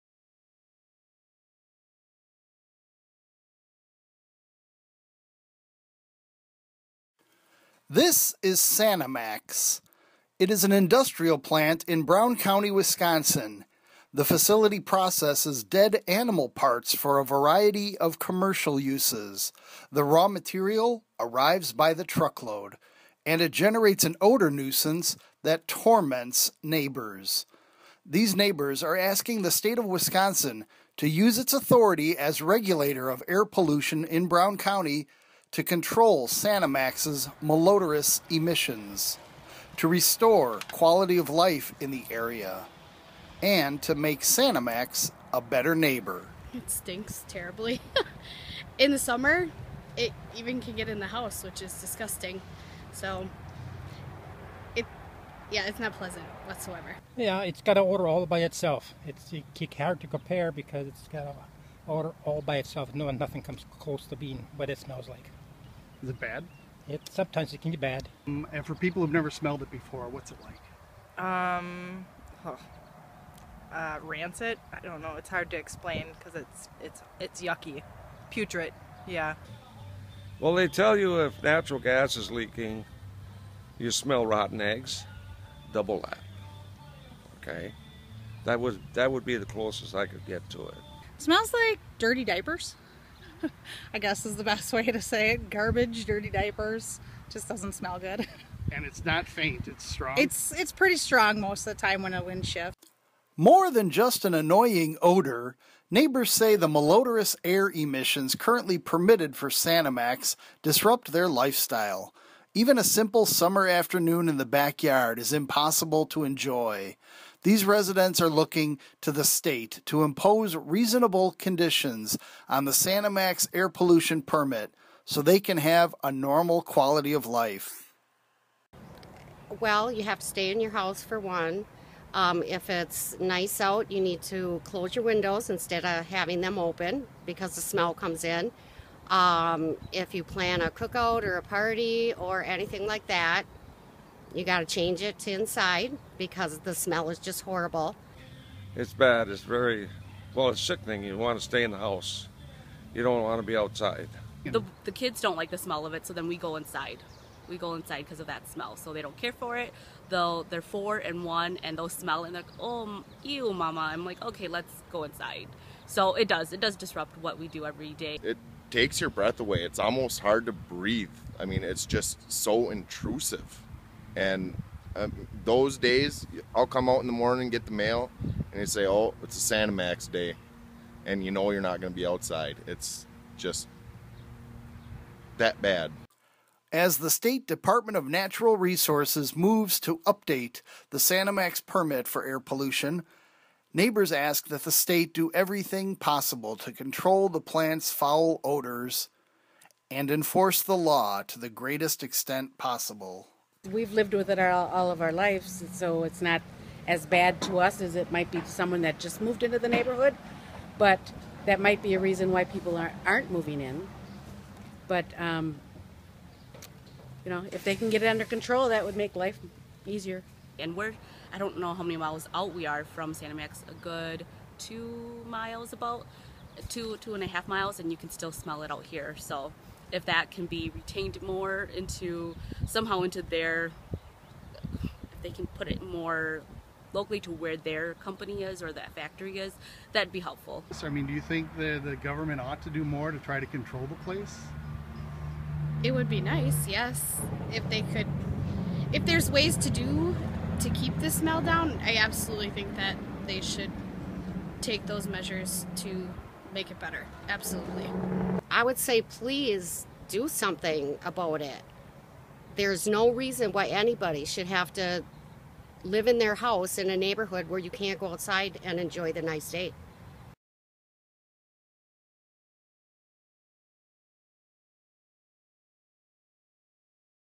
Sanimax neighbors plead for relief from stench
As state regulators get tough on the Sanimax factory's foul odor pollution in Howard, Wisconsin, neighbors talk about how the nuisance has made life miserable.